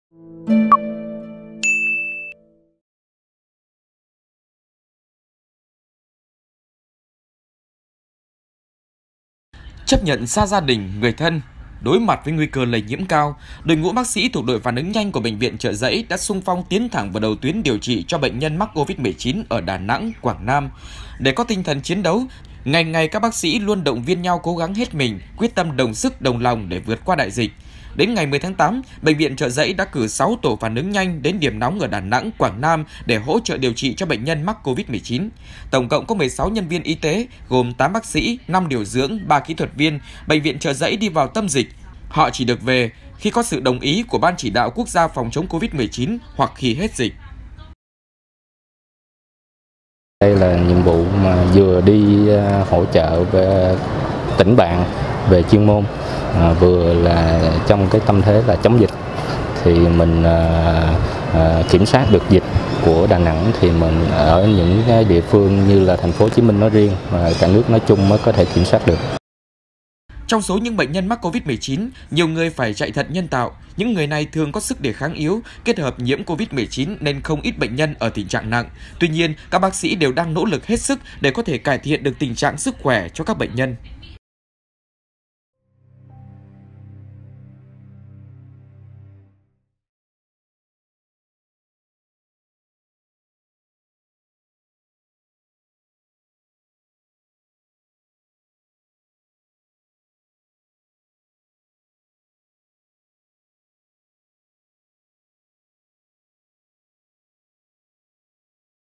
Bác sĩ Chợ Rẫy tại "điểm nóng" Covid-19: Ngày hết dịch sẽ là lúc trở về
Chấp nhận xa gia đình, người thân, đối mặt với nguy cơ lây nhiếm cao, đội ngũ bác sĩ thuộc đội phản ứng nhanh của Bệnh viện Chợ Rẫy đã xung phong tiến thẳng vào đầu tuyến điều trị cho bệnh nhân mắc Covid-19 ở Đà Nẵng, Quảng Nam.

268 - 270 Nguyễn Đình Chiểu, Phường 6, Q.3, TP HCM